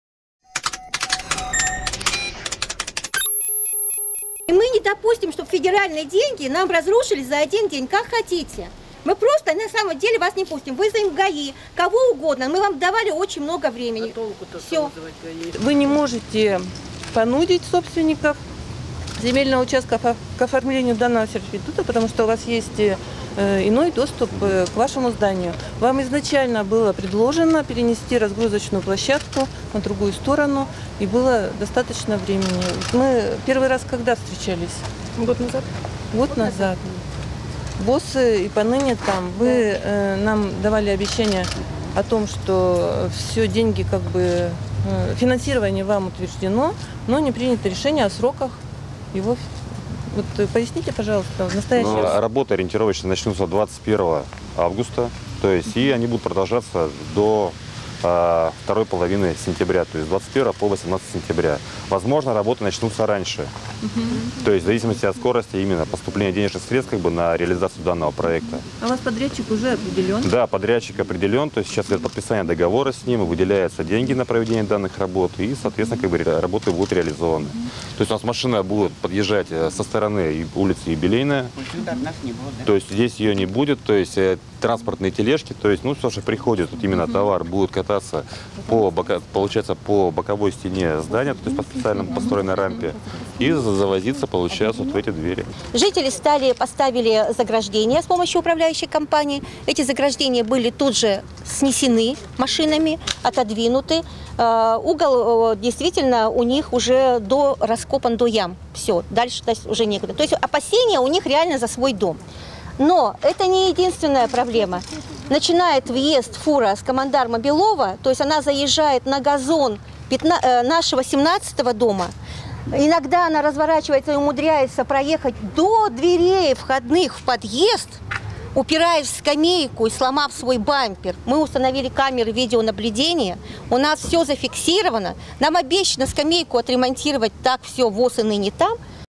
Конфликт жильцов и "Магнита"
Жители череповецкого дома заставили «Магнит» прекратить движение грузовиков через двор